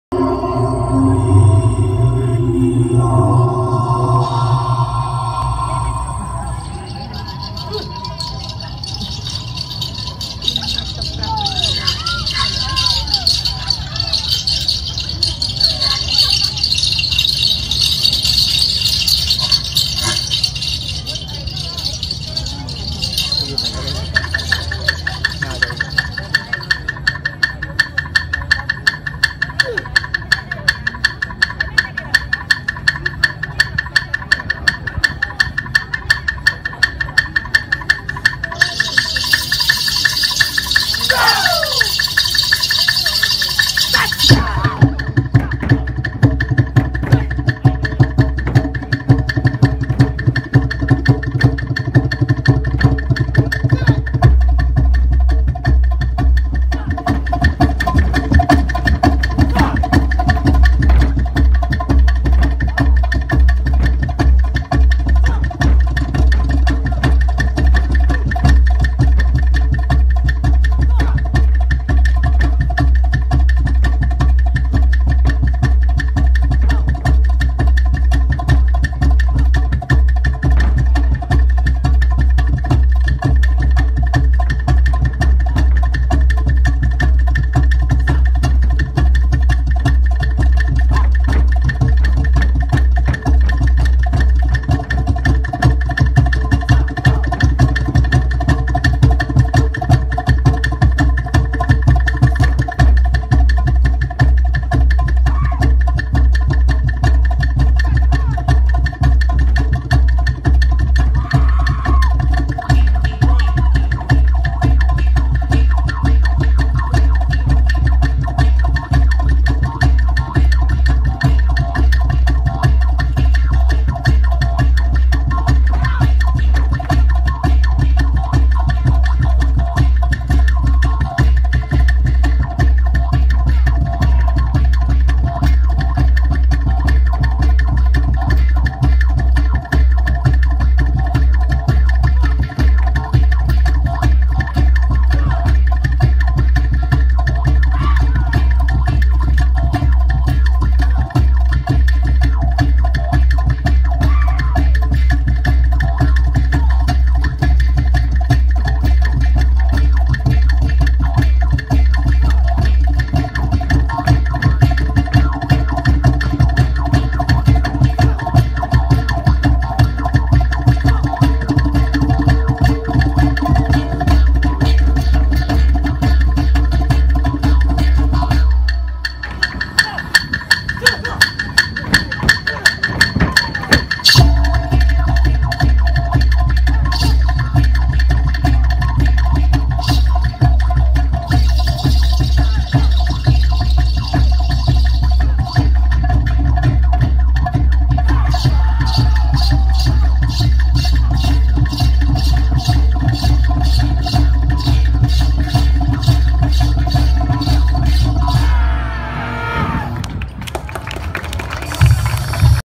CULTURAL PRESENTATION: “Ta'rek” | Batak (IPs of Palawan) War Dance
Ta’rek is a Batak War dance played during lambay ritual.

This video was taken during the cultural night held at Puerto Princesa City Coliseum.